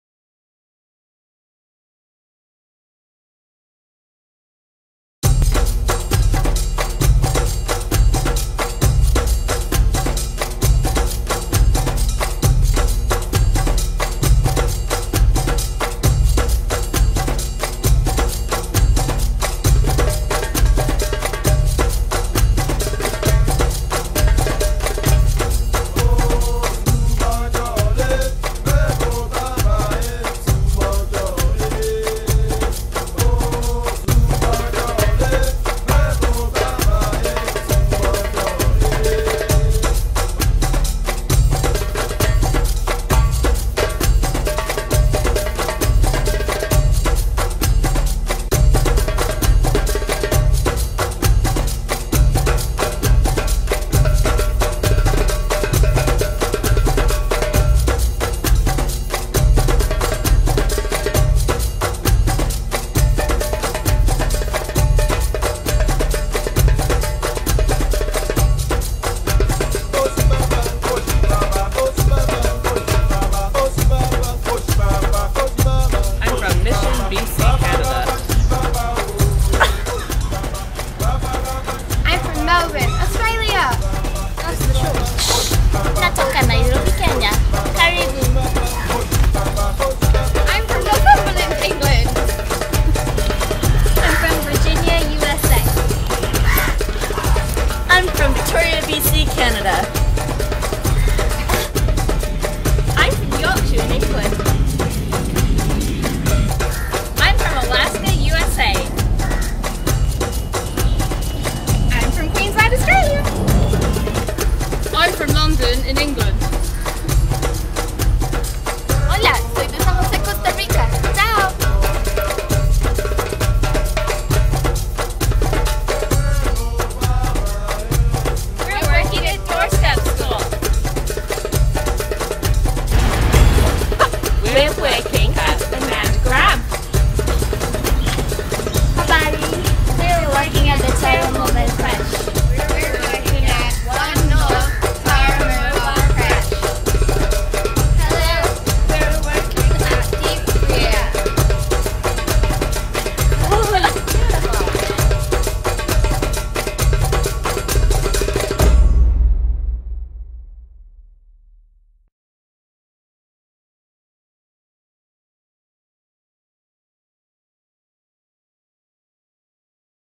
Tare Welcome
Gosh our Tare are talented! They have made this fab video to tell you all who they are, where they are from and where they are working while they are at Sangam!

If you think you would make a great Tare, we want to hear from you! You can apply to be on the programme